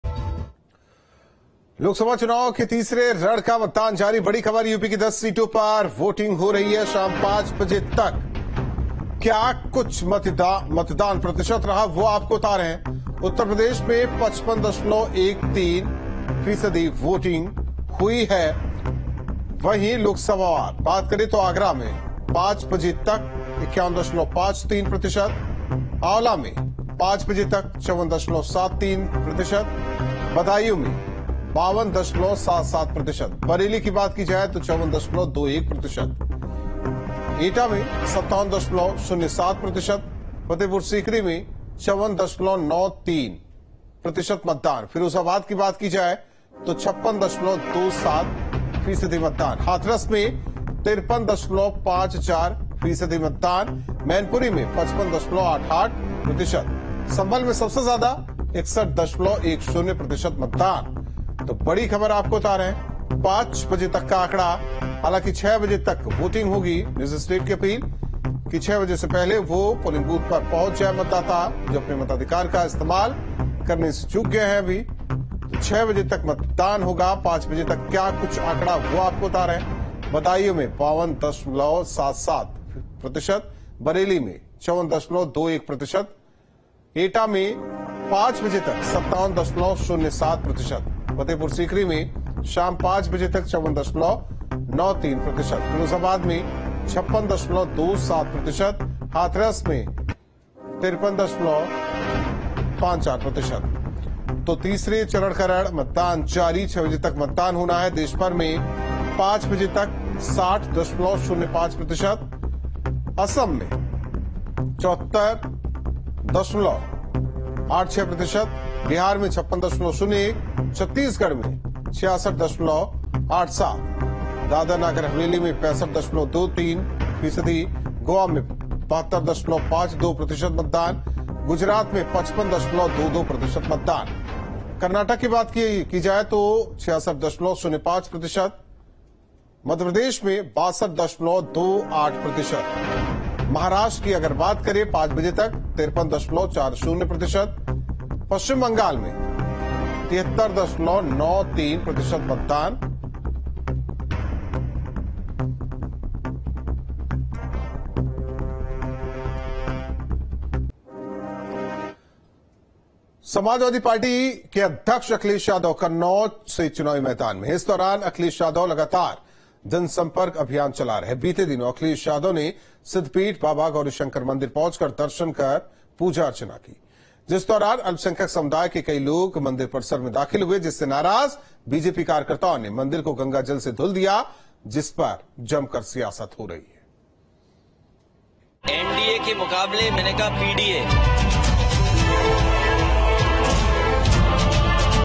Lok Sabha Election 2024 Phase 3 Voting Live Updates: शाम 5 बजे तक कहां हुआ कितने फीसदी मतदान
News State Apps डाउनलोड करें:
Download Android and iOS Apps for more updates: